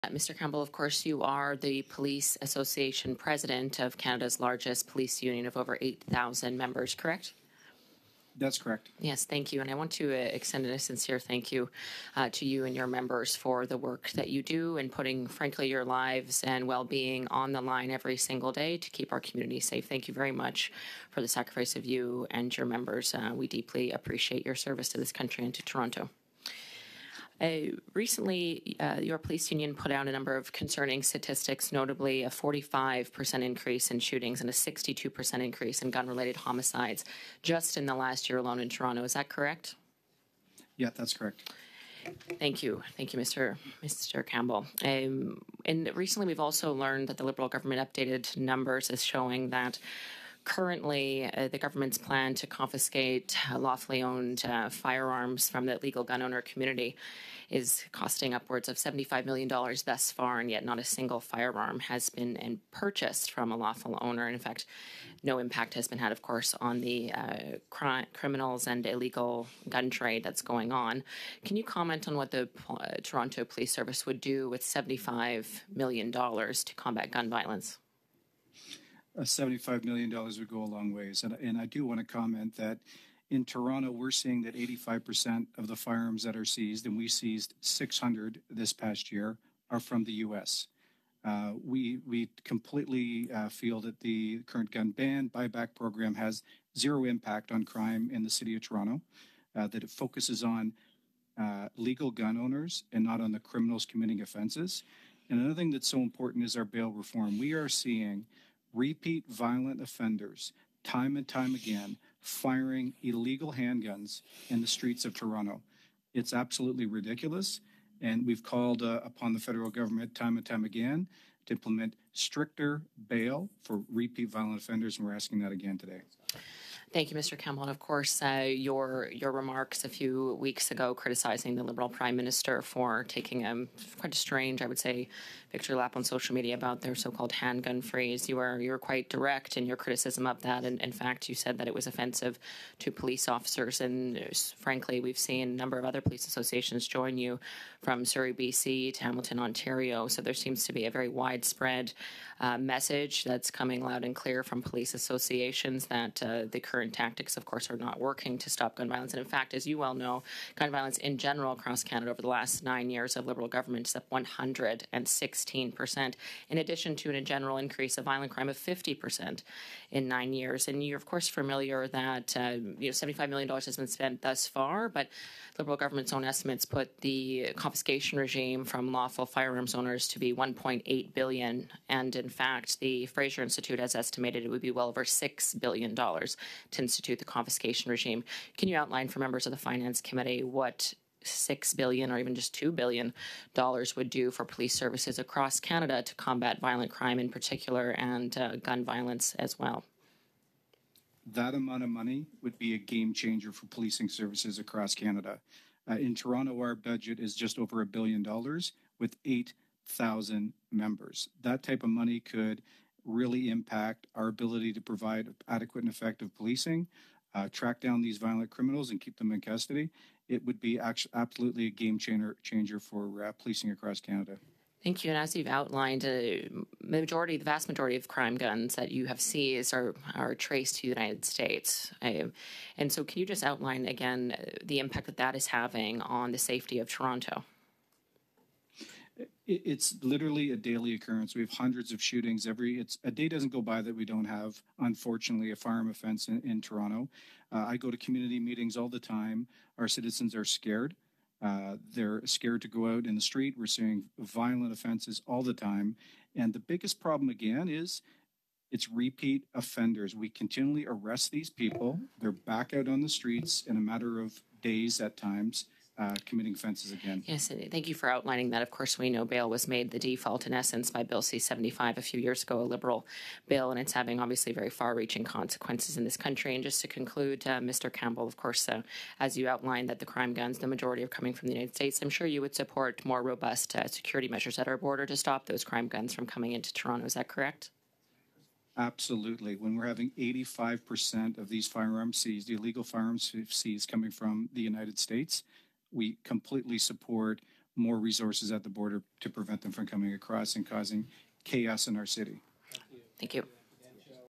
Police Expose Liberal Crime Record
Police are speaking out: Justin Trudeau's policies on crime and justice are a failure and Canadians are paying the price.

Repeat violent offenders are going free on bail often the same day they are arrested.

Criminals are smuggling guns across our borders with impunity while the government wastes billions taking firearms from law-abiding Canadians.

Common sense Conservatives will give repeat offenders jail, not bail, and will scrap the wasteful gun confiscation and redirect those funds to our frontline police officers.